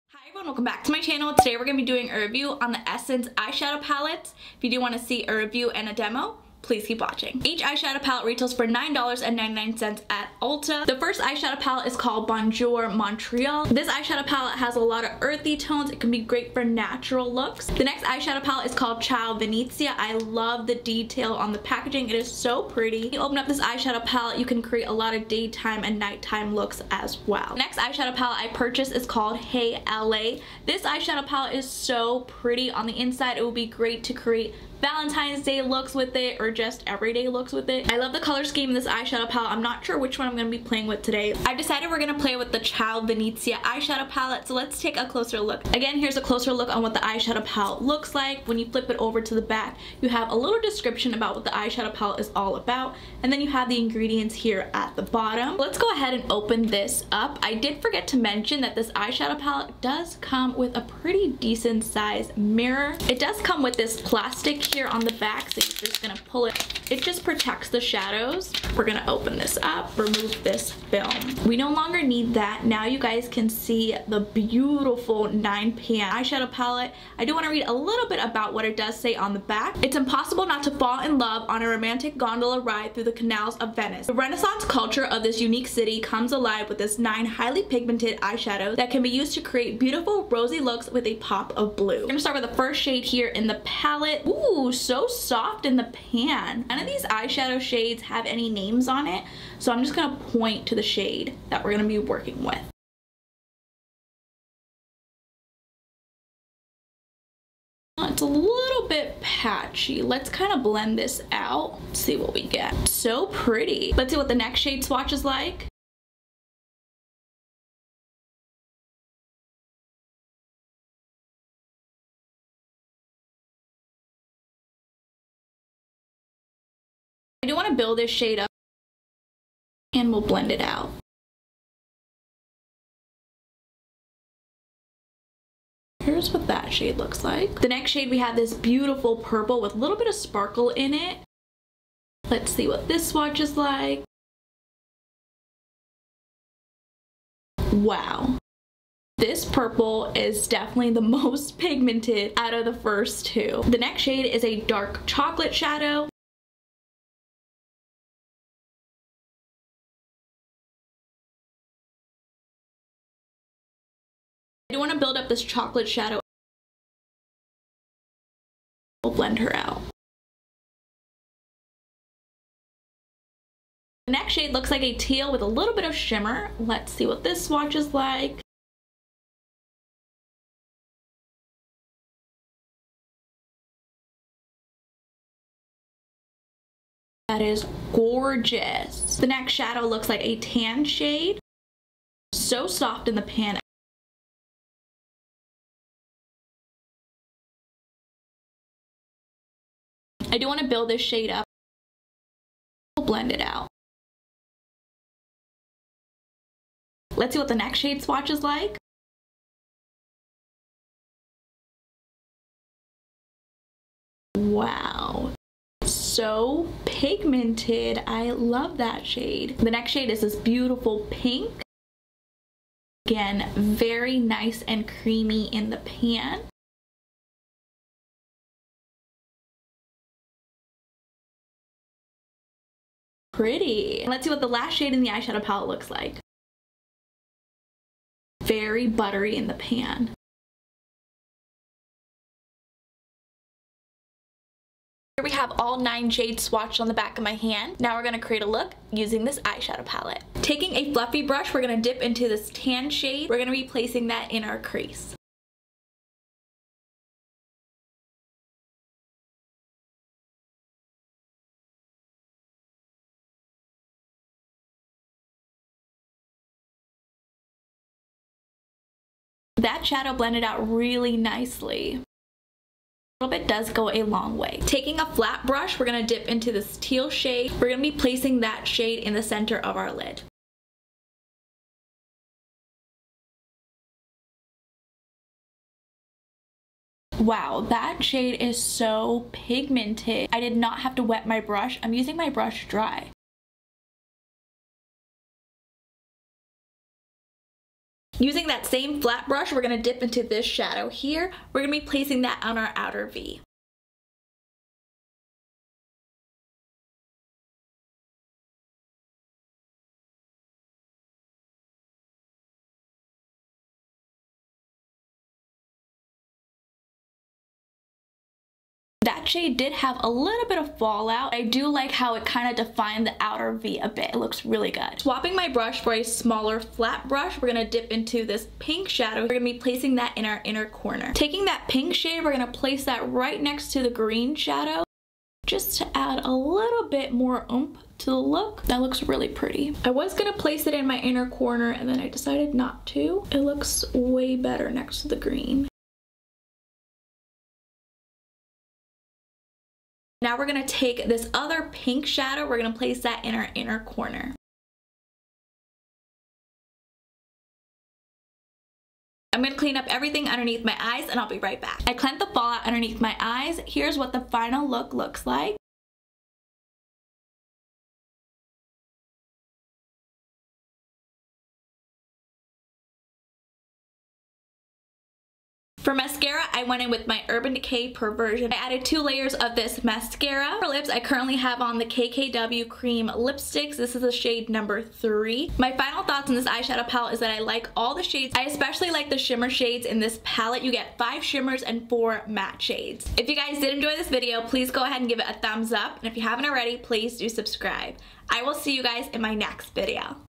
Essence Ciao Venezia Eyeshadow Palette Review
This palette has 9 shades, It retails for $10 at UIta Beauty. I hope you all enjoyed this review on the Essence Ciao Venezia Eyeshadow Palette.

xoxo Candace

Essence Ciao Venezia Eyeshadow Palette:

I use MagicLinks for all my ready-to-shop product links.